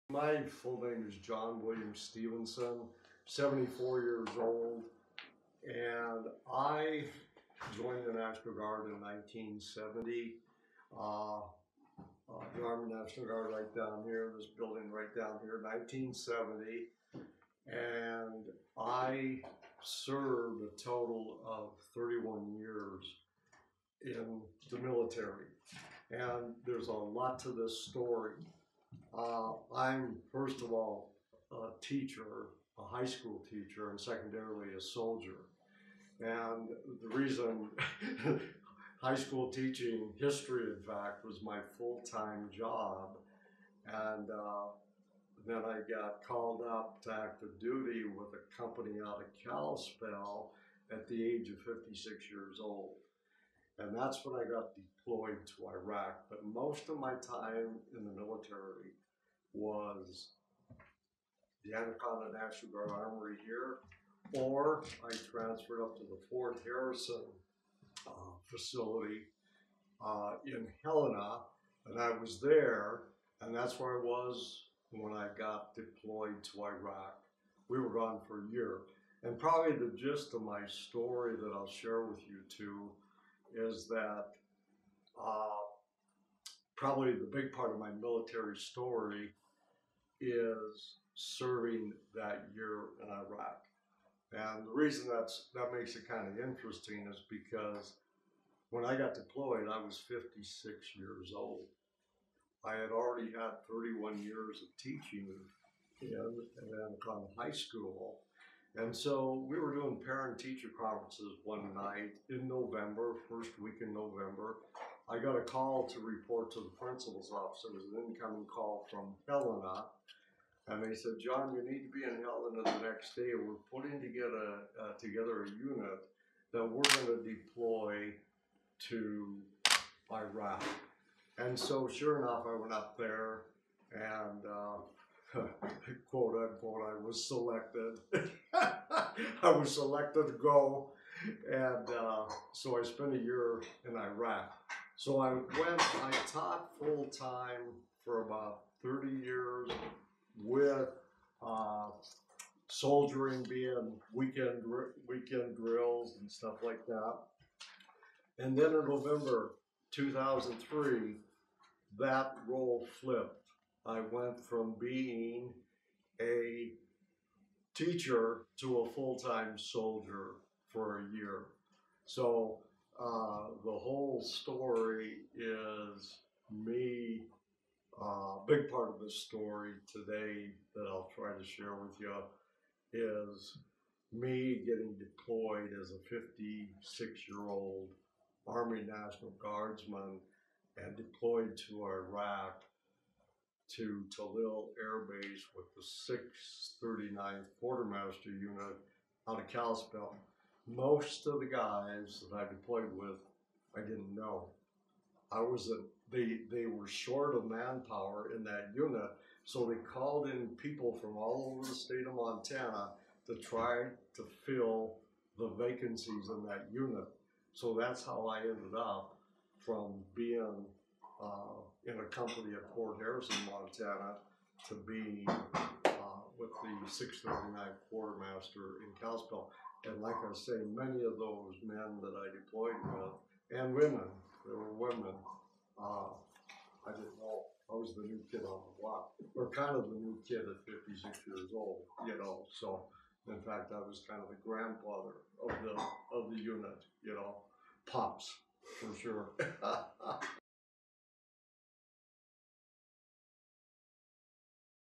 Stories of Honor: John Stevens
John Stevens, of Anaconda, is interviewed for a feature on Tuesday, June 7, 2022. Stevens served in the US Army for 31 years.